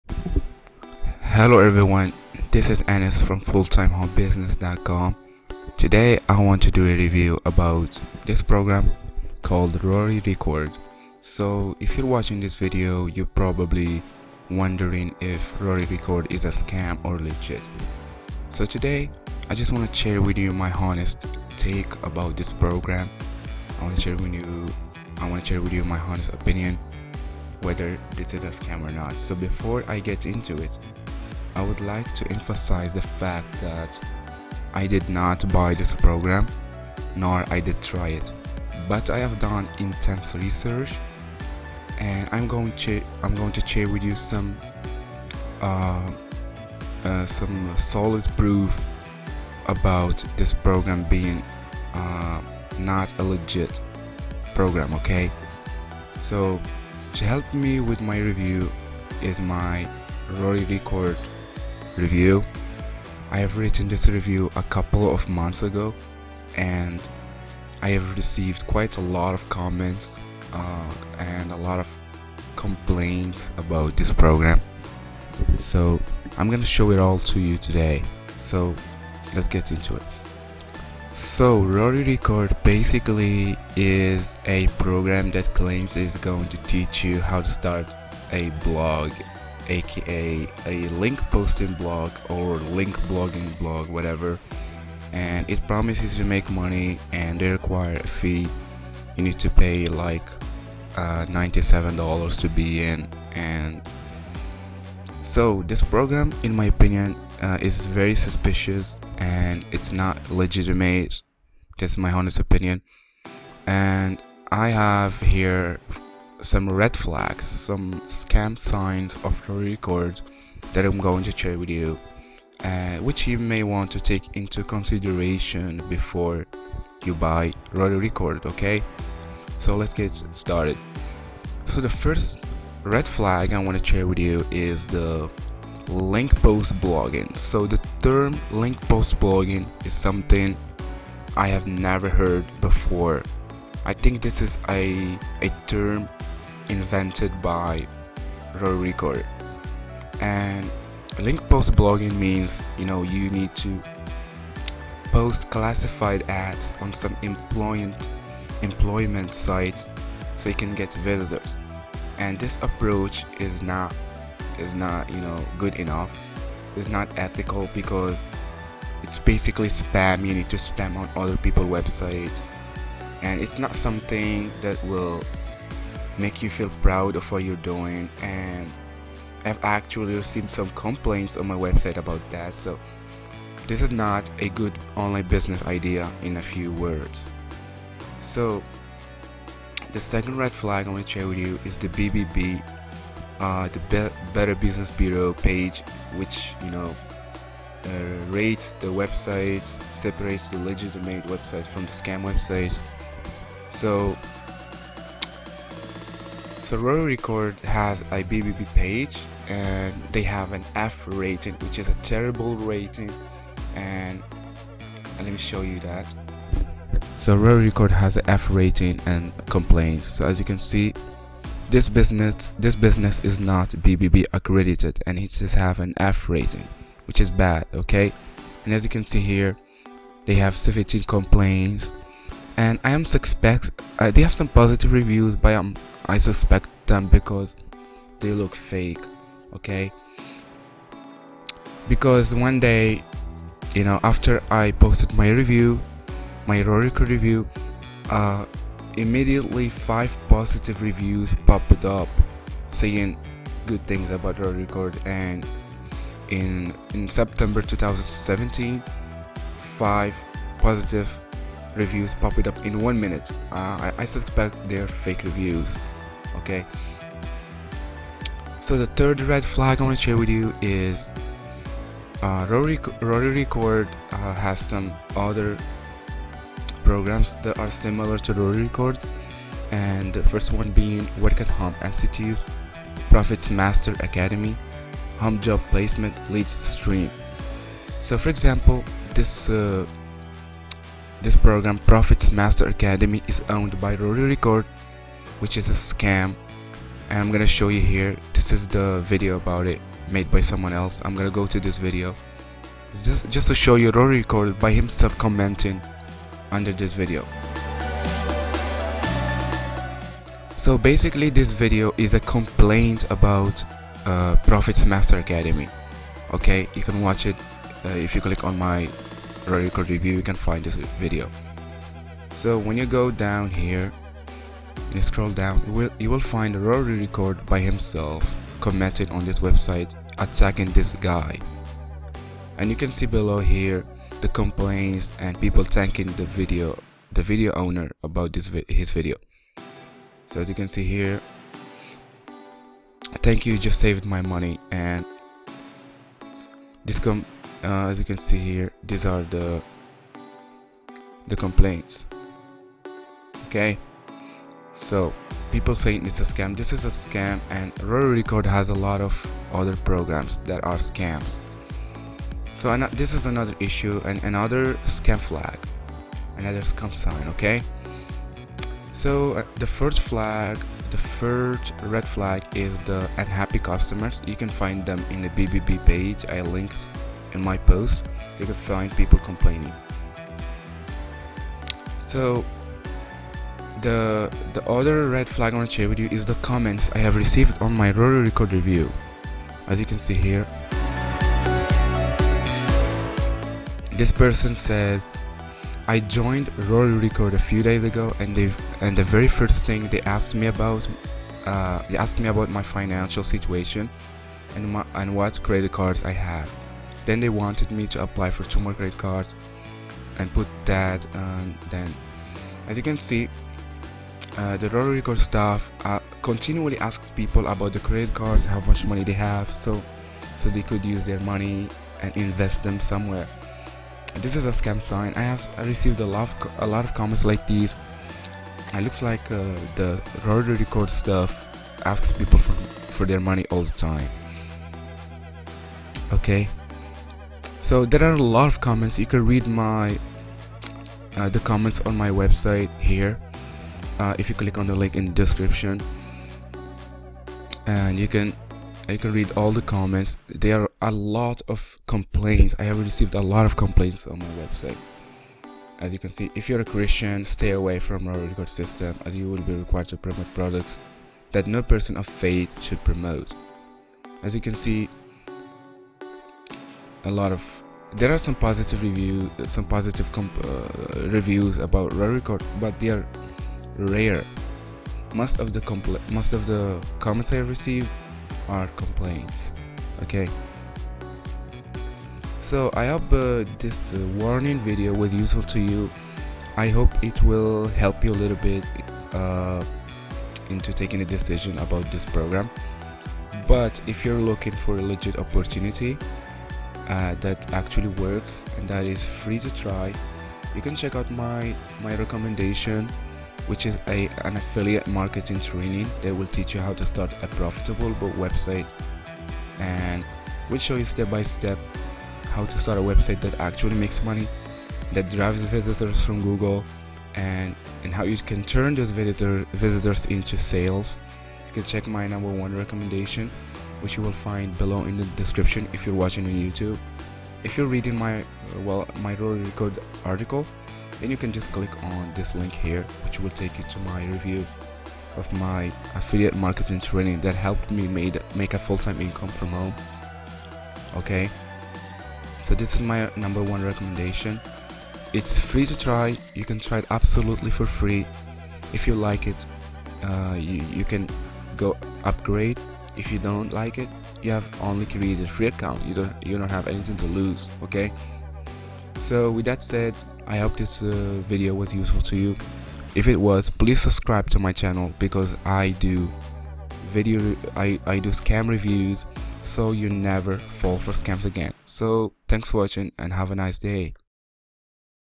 Is Rory Ricord a Scam? 2019 Review Exposes this FRAUD!
Is Rory Ricord a Scam? Or Legitimate Training?

I want to say that I did not buy Rory Ricord

In this review, I only share with you my opinion and my findings of Rory Ricord

In this Rory Ricord scam review, I share with you different red flags that forced me not to recommend this program.

I have updated my Rory Ricord review, that's why you will see the Rory Ricord review I talk about in the video is different than the one currently published, just click on the link above to read my 2019 updated Rory Ricord review!